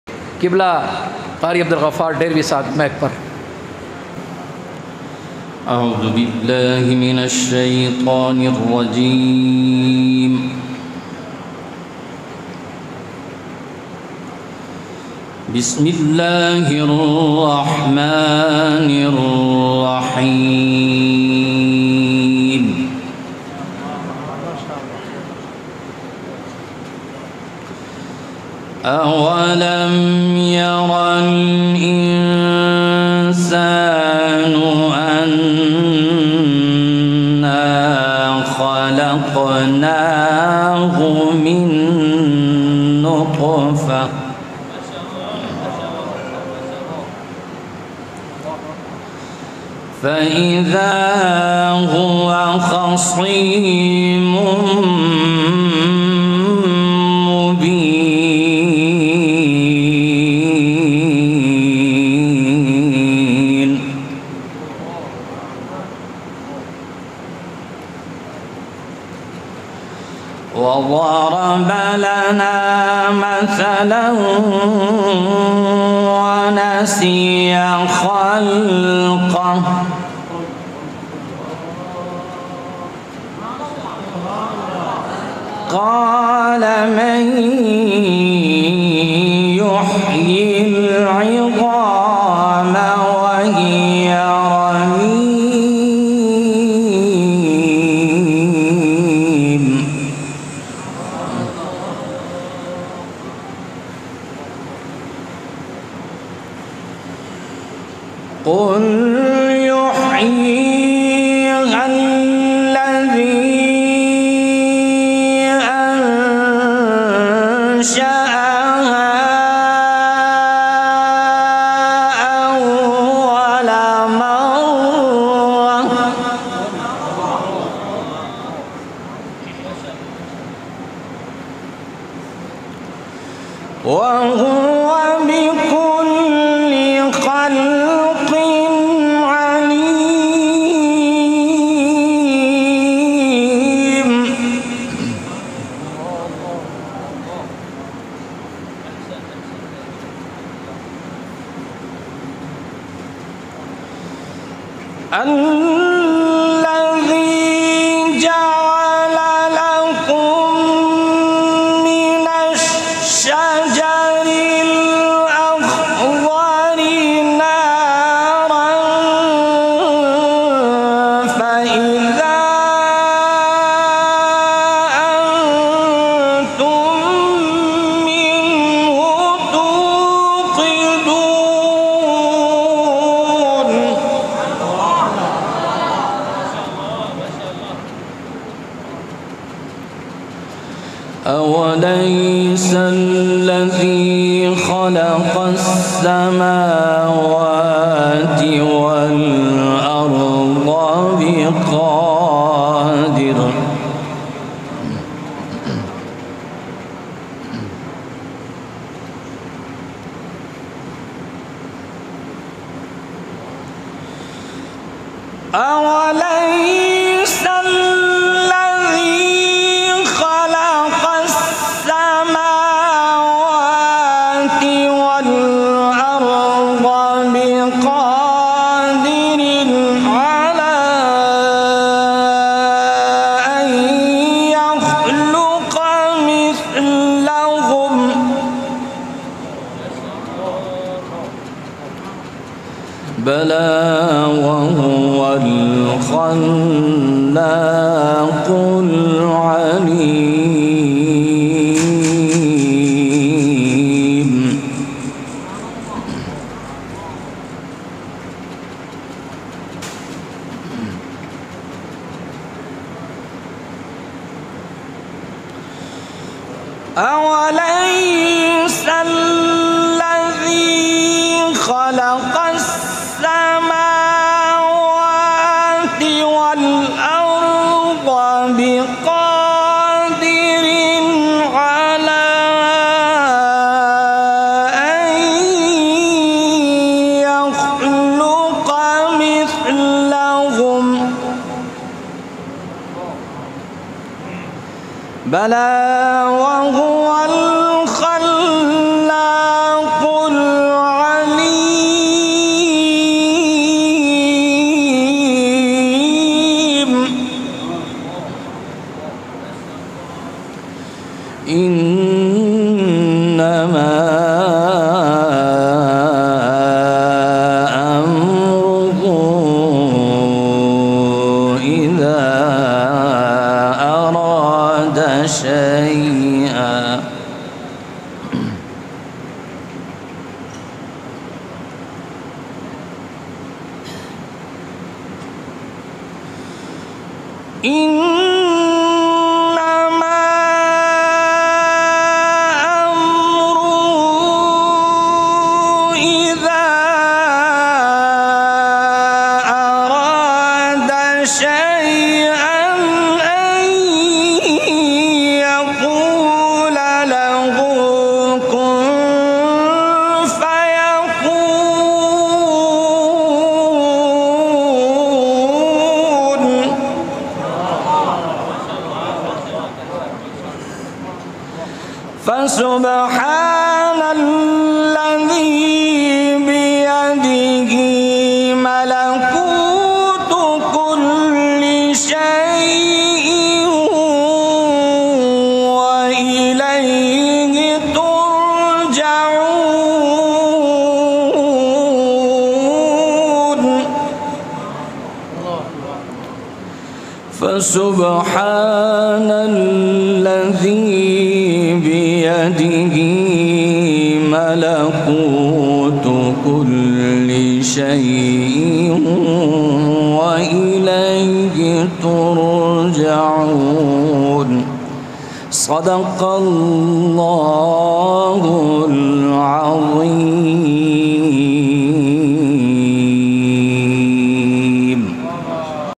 Tilawat || Qari Abdul Gaffar Dervi || JAMIA SIDDIQUIA BAHAWALPUR || تلاوت || قاری عبد العفار ڈیروی
JAMIA SIDDIQUIA BAHAWALPUR 4 AGUST 2021